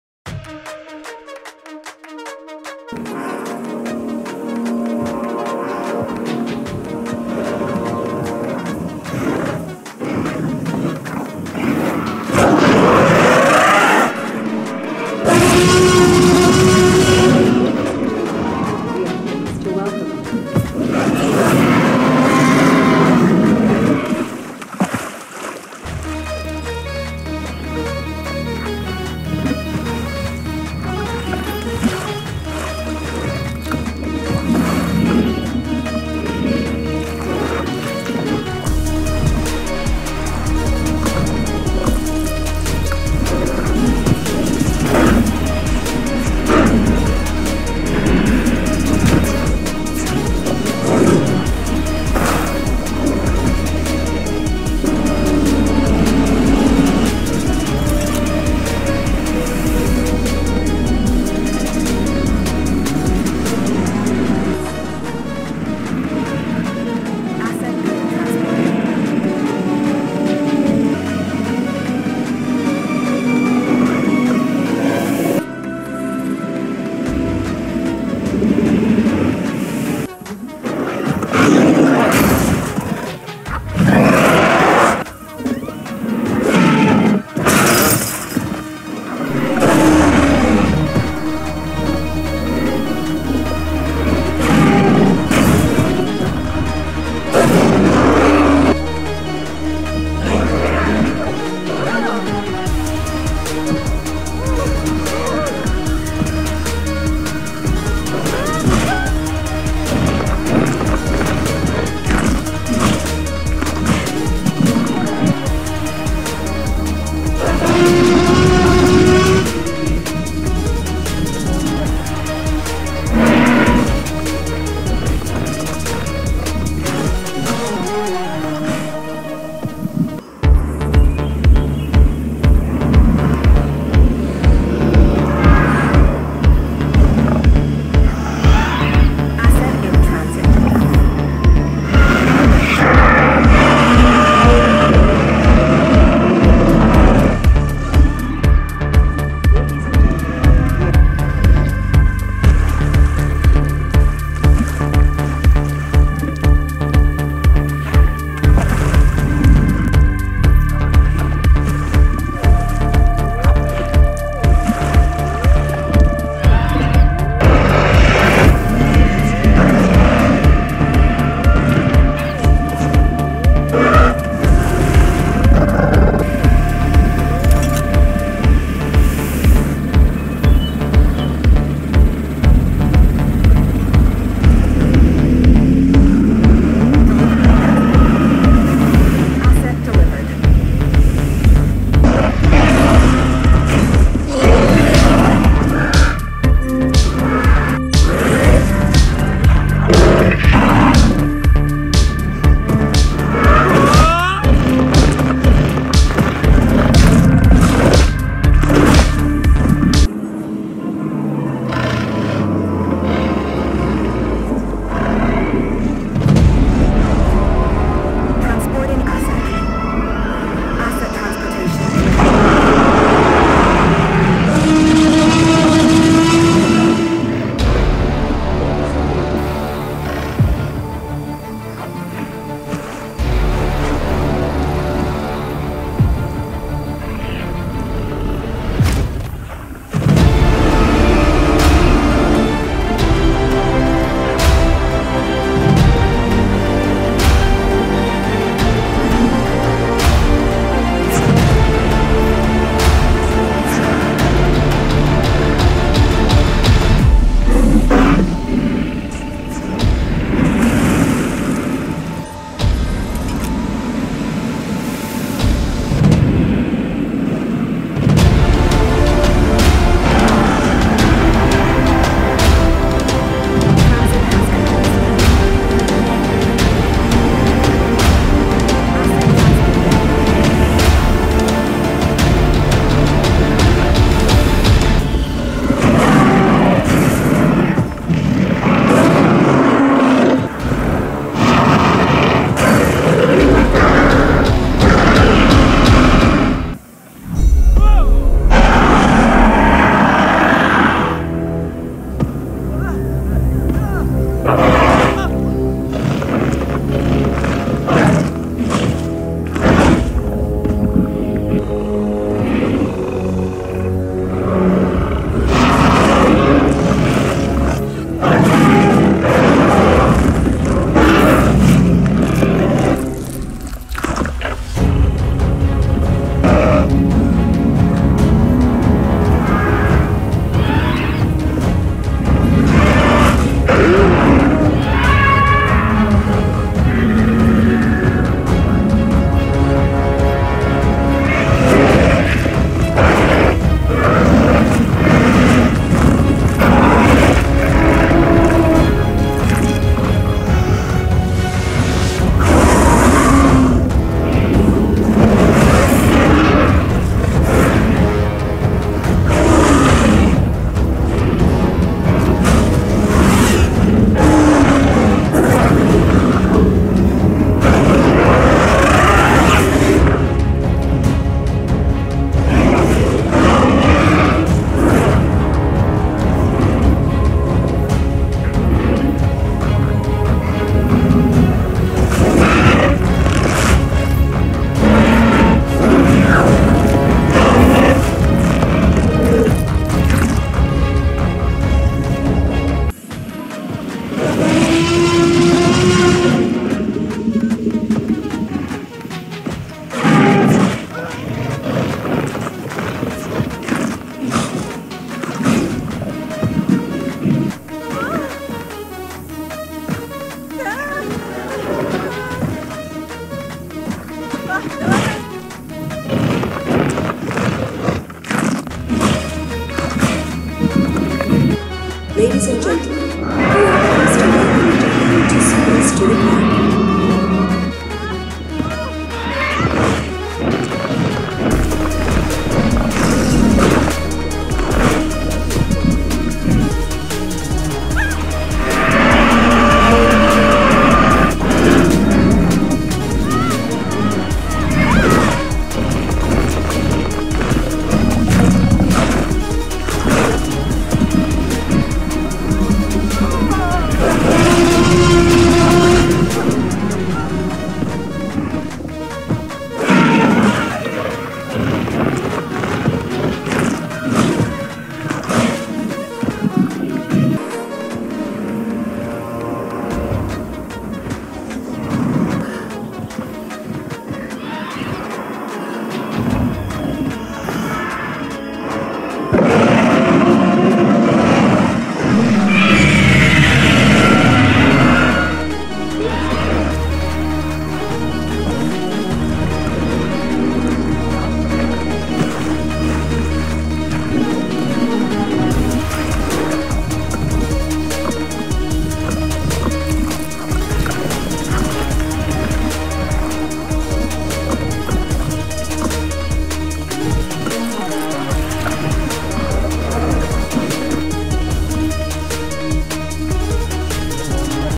ALL GREEN Giant Colour Pack Super Hero Dinosaurs BATTLE : Spiderman, Hulk, Carnage, G Goblin, Grinch
GREEN SPIDER-MAN GIGANOTOSAURUS
GRINCH CARNAGE
GREEN SHE HULK I-REX
GREEN SPIDERMAN MONSTER SPINOSAURUS
GREEN GOBLIN !
SPIDERMAN GREEN PACK TREX

Best and Powerfull Spider-man Dinosaurs Attacked by a helicopter and make some trouble in Jurassic World Universe creatures.